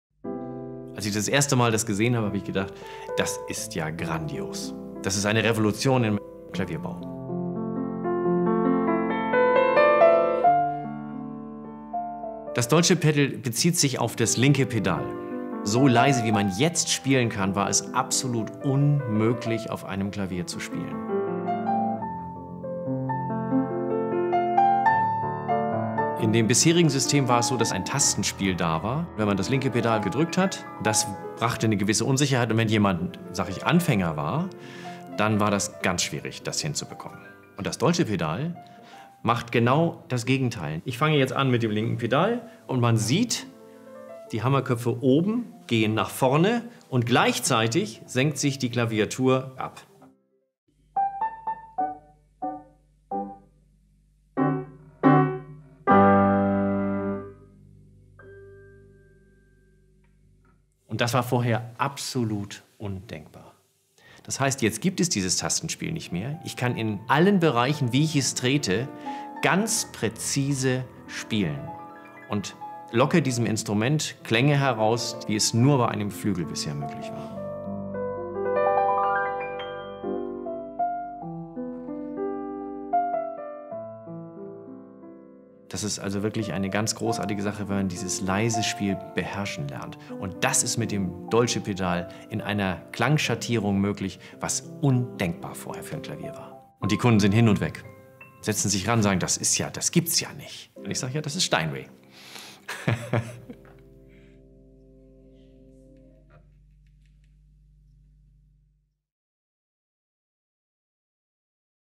Steinway Dolce Pedal
For a pianist looking for the perfect upright piano.

The dolce pedal on a Steinway upright allows you to play with perfect precision and nuances, creating sound that you normally only achieve with a grand piano.

Available in Steinway Upright K-132 and Steinway Upright V-125, it is the perfect instrument for uncompromising musical pleasure when space is too tight for a grand piano.

Visit us at Level 4, ION Orchard and experience it for yourself.